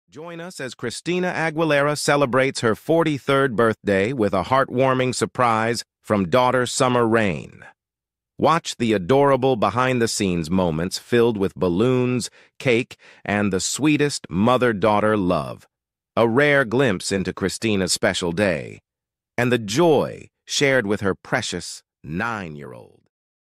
Christina Aguilera's Sweet Birthday Surprise from Daughter Summer! 🎂🎈
Join us as Christina Aguilera celebrates her 43rd birthday with a heartwarming surprise from daughter Summer Rain!  Watch the adorable behind-the-scenes moments filled with balloons, cake, and the sweetest mother-daughter love. A rare glimpse into Christina's special day and the joy shared with her precious 9-year-old!

Video Voice                     -  Voice From Elevenlabs.io website
Video Information          - From Google
Video/Image Present    - Googles & Instagram